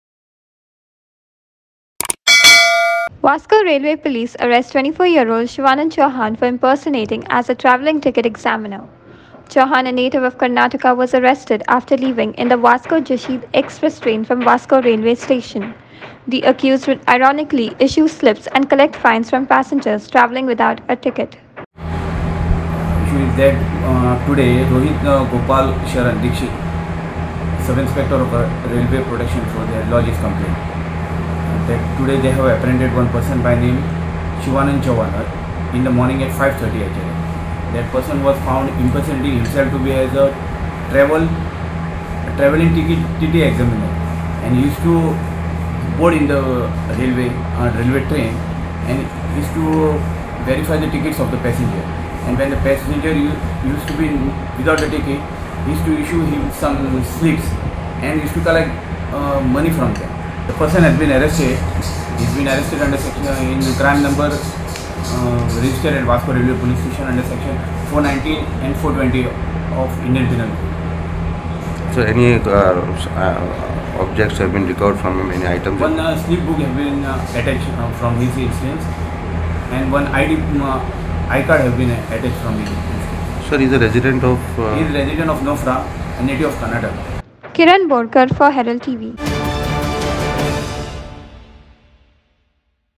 One arrested for impersonating a Ticket Examiner at Vasco railway station
Vasco Railway police arrested 24-year-old Shivanand Chauhan for impersonation as a Travelling Ticket Examiner. Chauhan, a resident of Nofra and a native of Karnataka was arrested after leaving in the Vasco Jasidih express train from Vasco Railway station. Police recovered a slip book and an ID card from the accused. The accused would ironically issue slips and collect fines from passengers travelling without a ticket. The accused is arrested under IPC sections 419 and 420.